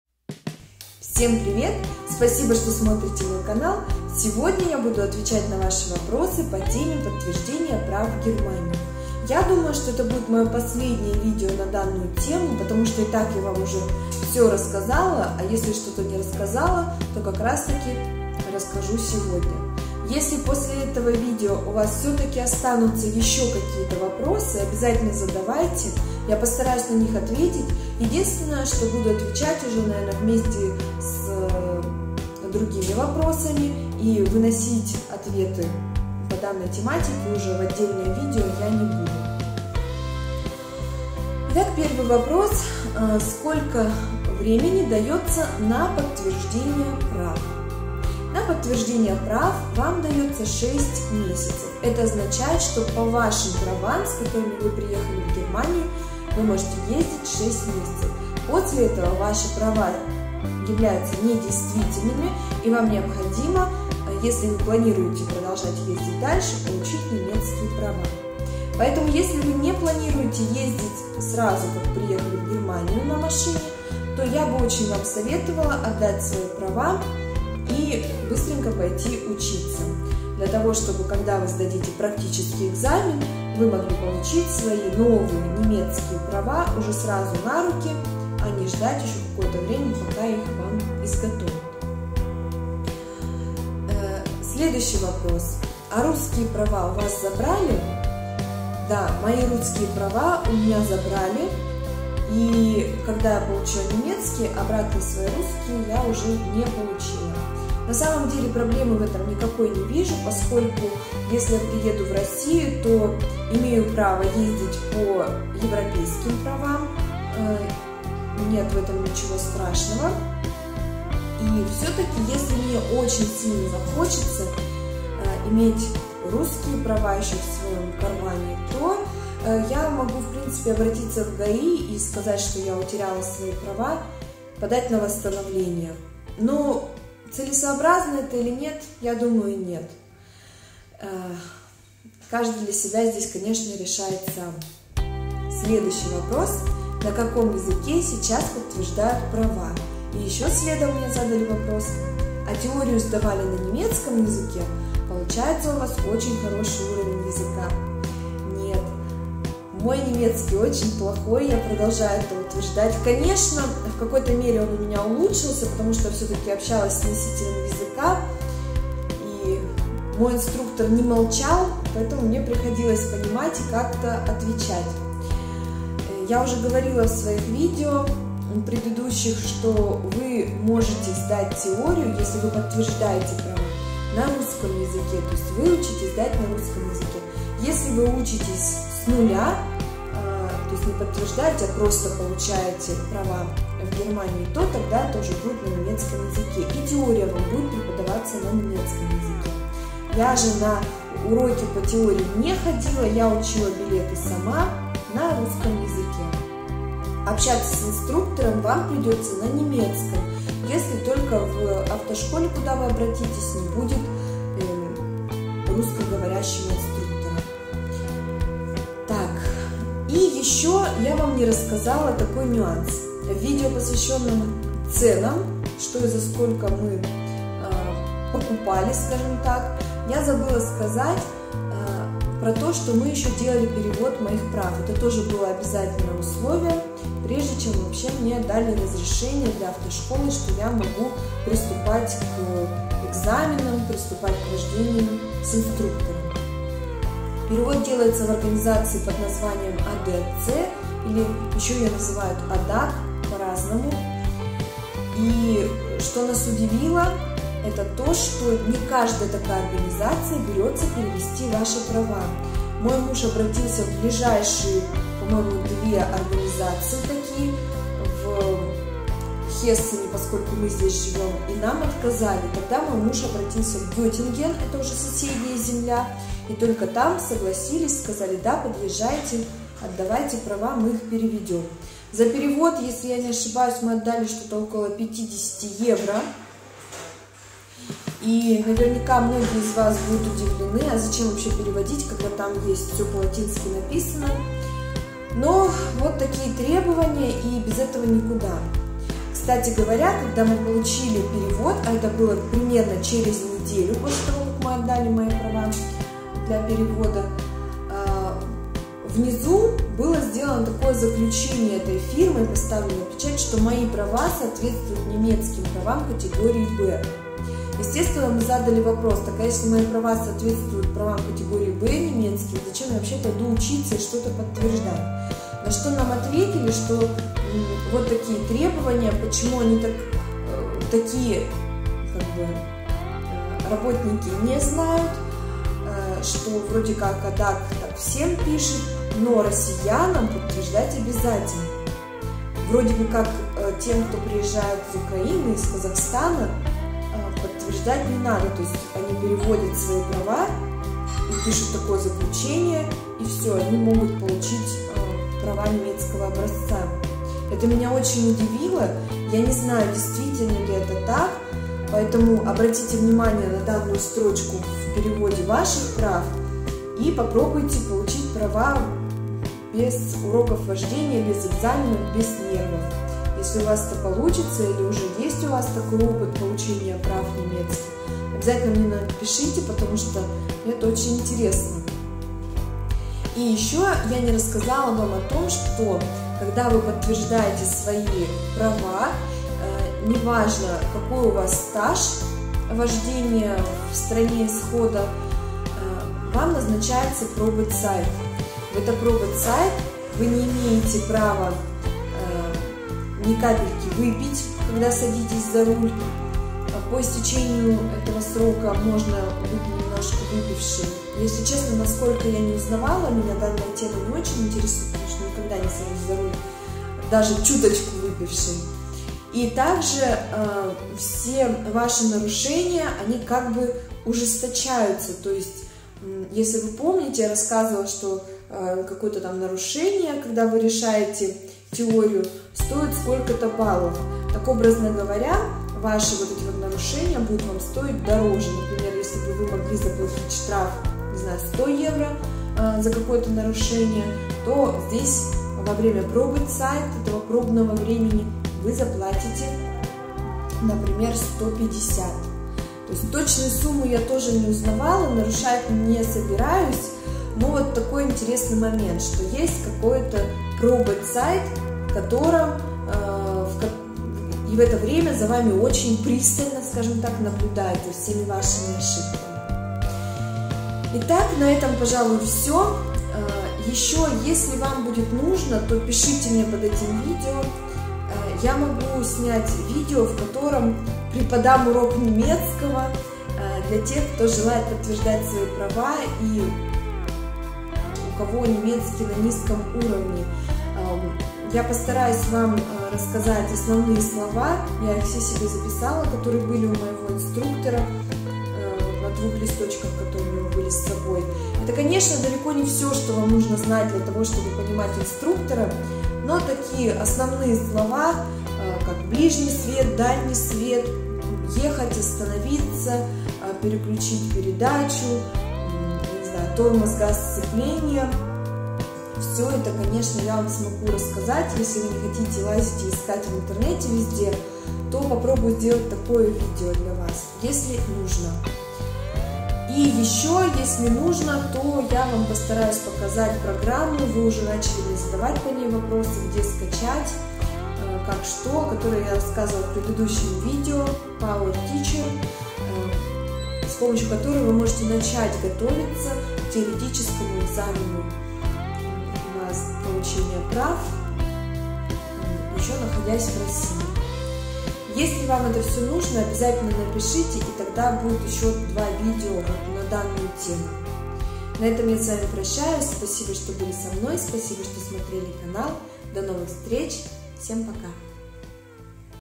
Ответы на ваши вопросы: подтверждение прав в Германии
Всем привет!
В этом видео я отвечаю на ваши вопросы по теме подтверждения прав в Германии.
Я в Instagram Saschalisa55